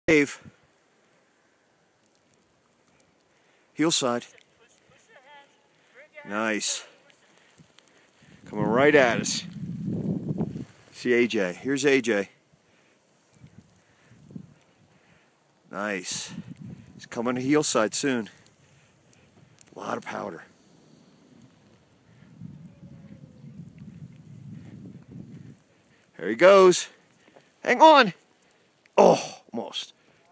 Aspen snowmass snowboarding 4
Snowboarding with Toshi in Aspen 2012